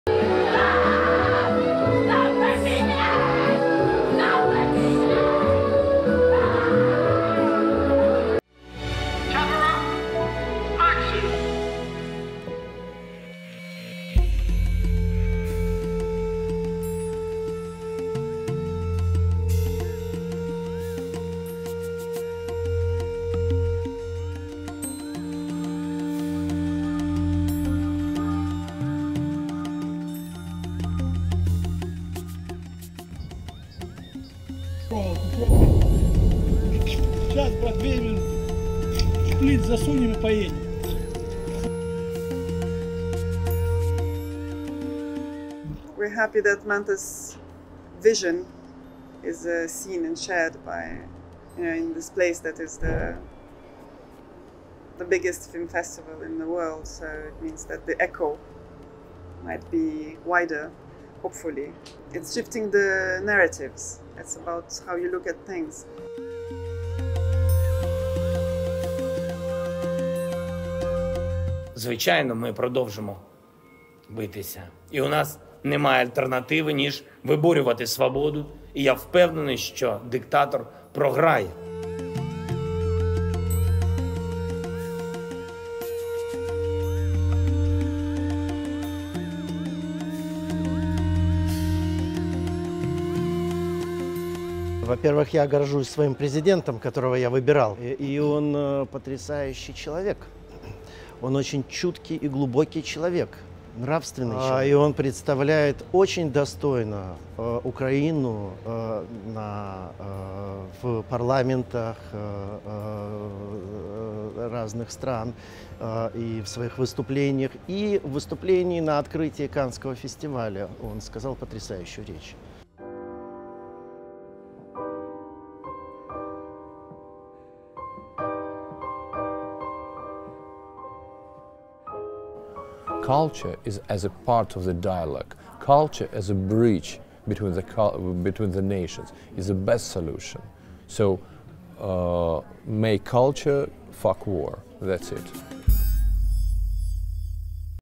'Make culture, f*** war’: Ukraine takes starring role in Cannes • FRANCE 24 English
The war in Ukraine took a starring role on the opening night of the 75th Cannes Film Festival and it has rarely been far out of frame since. Throughout the French Riviera spectacular has run a discourse about the role of cinema in wartime. FRANCE 24’s Nina Masson and Juliette Montilly report.